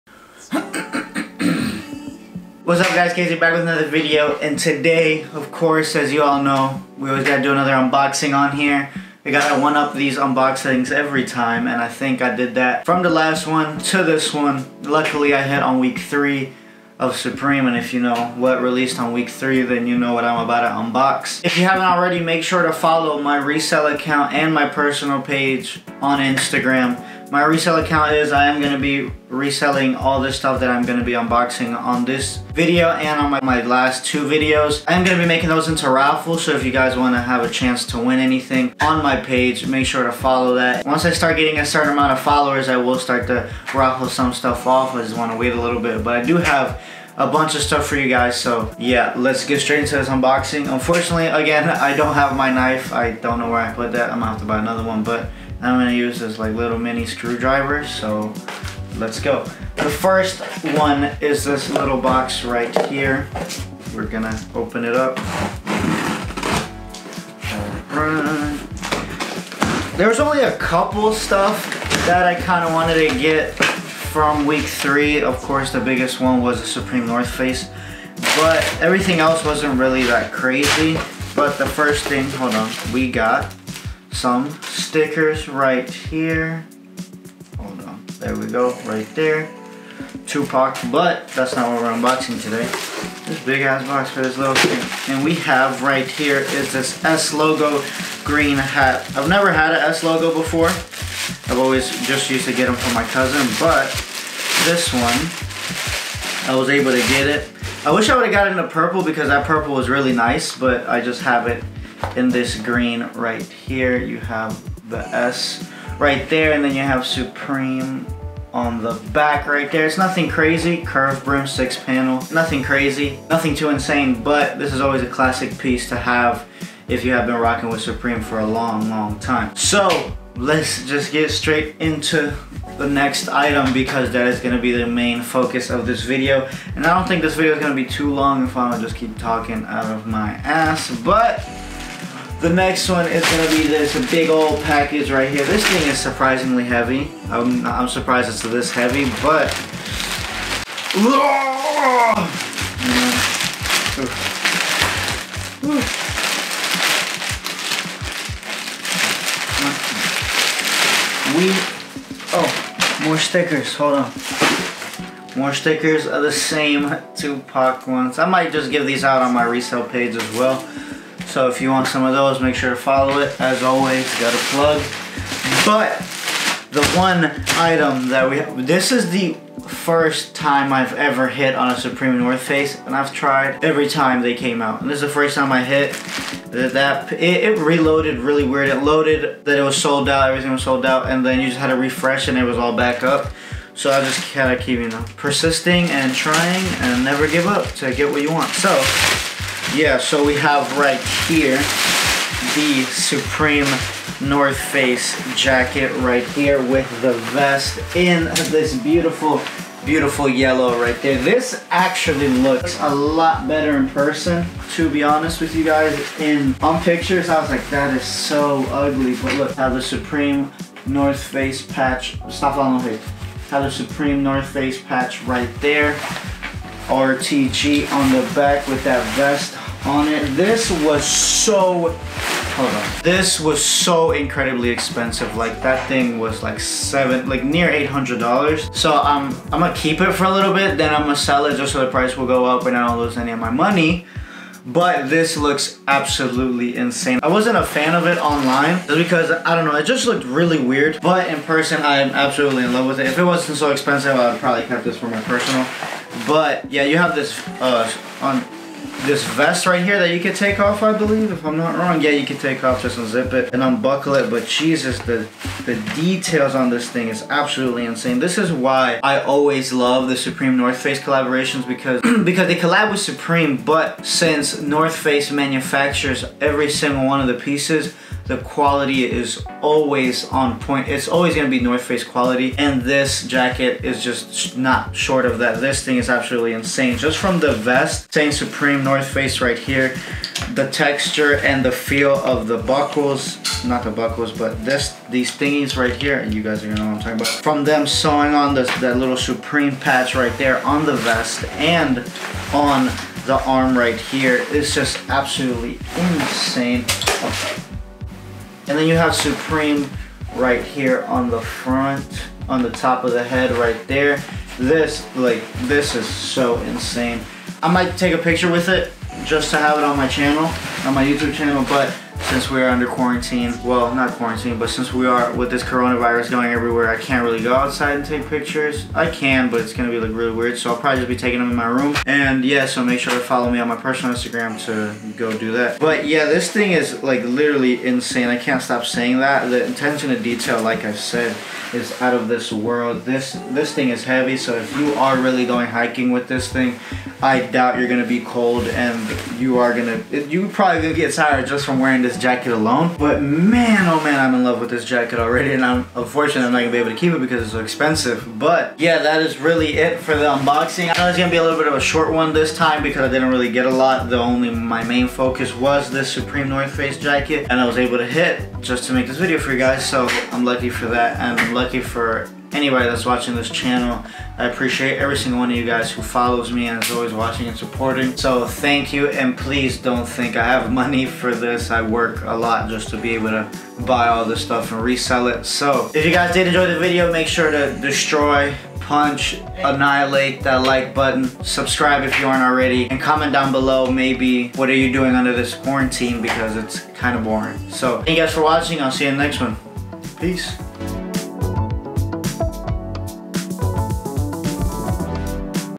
THIS JACKET IS STRAIGHT FIRE - SUPREME X TNF UNBOXING SS20
Oh BOY is this unboxing short but is it fire. Jacket is crazy and I hope you enjoy this little unboxing.